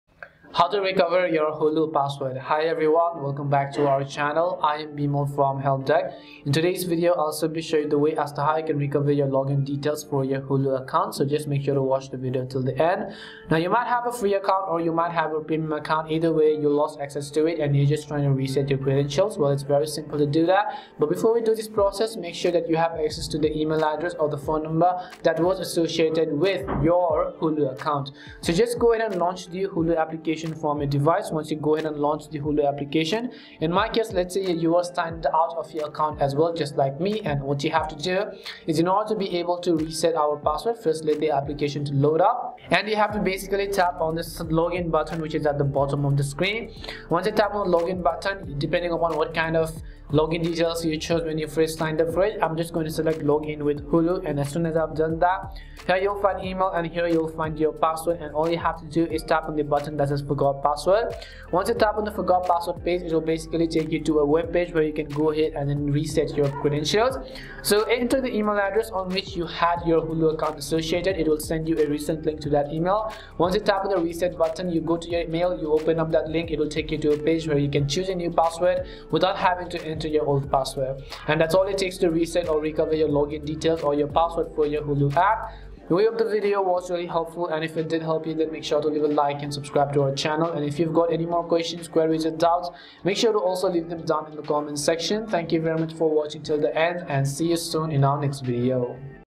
How to Recover Hulu Password !
This video guides you in quick easy steps to recover your Hulu password. So make sure to watch this video till the end.

1) Launch the Hulu app
2) Go to the login page
3) Here, tap on Forgot Password
4) Enter the email associated with your Hulu account
5) Enter a new password after verification
6) That's all it takes to recover your Hulu password.

~ Time Stamps:
0:00 Introduction
0:13 General Info
0:42 How to Recover Hulu Password
1:21 Conclusion